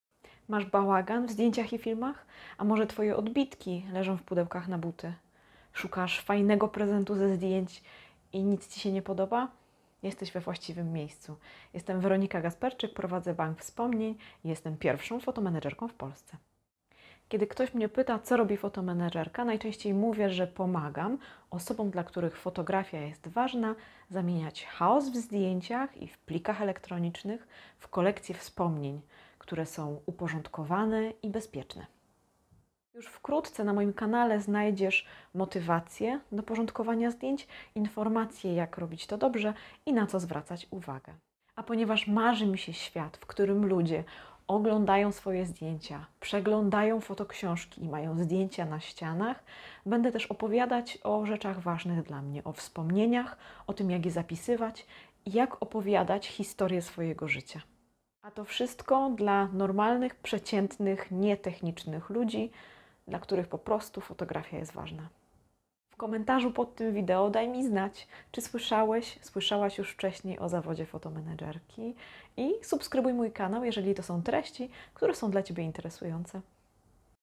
Zastanawiasz się, jak uporządkować zdjęcia i filmy? Rozgość się na moim kanale!
Cześć! Jestem Weronika.

Jako photo managerka pomagam ludziom, dla których fotografia jest ważna, porządkować ich zbiory zdjęć, aby mogli cieszyć się nimi każdego dnia. Robię to, ponieważ chcę ułatwić im dostęp do zdjęć i wspomnień utrwalonych na fotografiach.

Pierwszy raz poczułam, że to jest właśnie to, co chcę robić, kiedy prawie straciłam wszystkie moje zdjęcia. Mój smartfon zaliczył bliskie spotkanie z metalowymi schodami ruchowymi i niestety przegrał. Razem z nim przegrały jakieś zdjęcia moje i mojej rodziny. Niestety nigdy nie dowiedziałam się jakie.